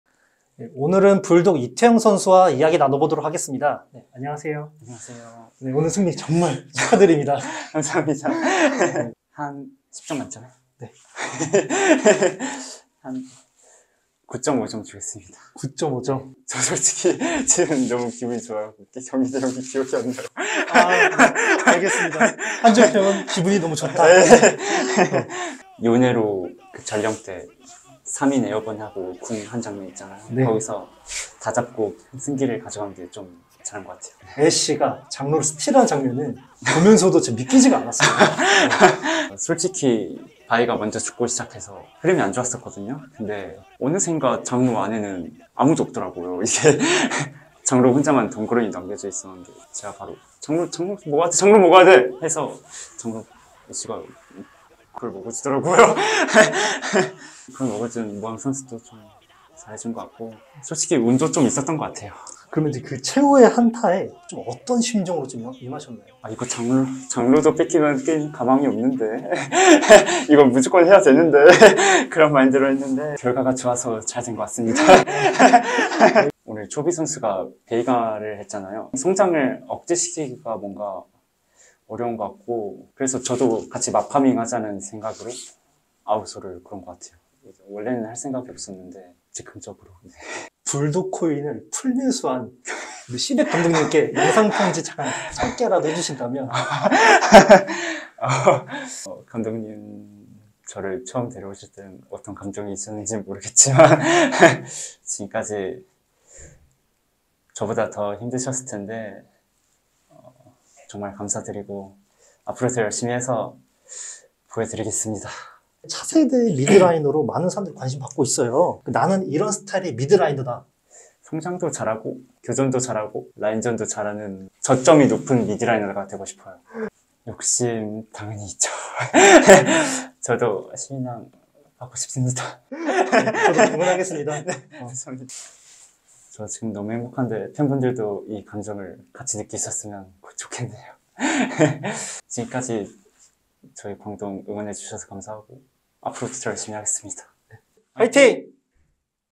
"저도..신인왕 하고 싶습니다" ['불독' 이태영] (2023 LCK 스프링 젠지 vs. 광동) / 이포커스 게임
0:00 인사
0:09 오늘 자신에게 주는 평점
0:25 오늘 가장 잘한 플레이
0:34 마지막 장로 스틸 장면
1:16 아우렐리온 솔 등장 배경은
1:30 감독님께 영상 편지
1:55 나는 이런 스타일의 미드라이너다
2:06 신인왕 욕심 날 만한데
2:15 끝으로 하고 싶은 말은

카카오톡 : '이포커스' 친구 추가 후 채팅